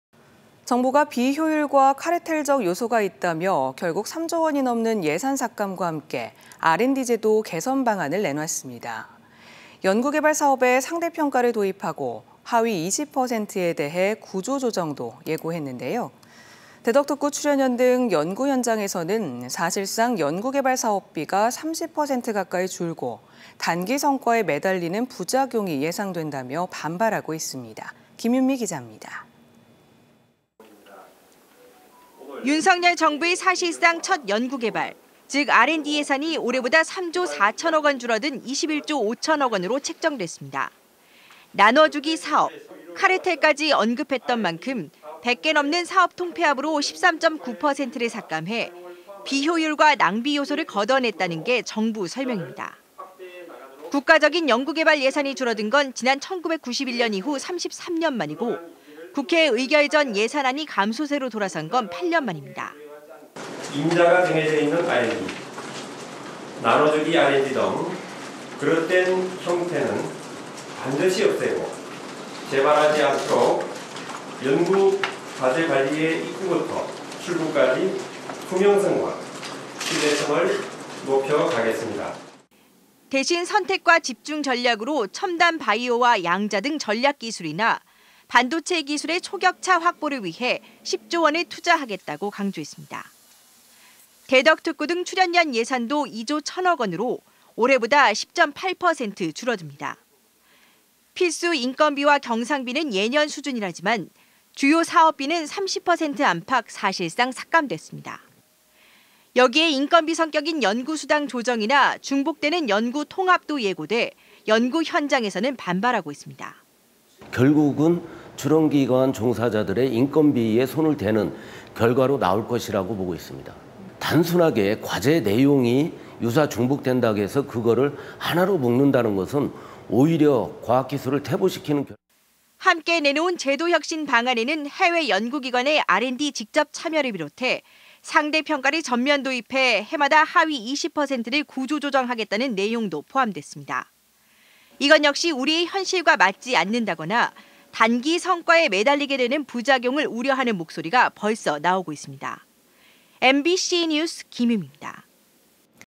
R&D 예산 삭감·구조조정 연구현장 '반발'/대전MBC
"3조 원 넘는 예산 삭감과 함께 R&D 제도 개선 방안을 내놓았습니다. 상대평가 도입과 하위 20%에 대한 구조조정도 예고해 연구 현장에서 반발하고 있습니다."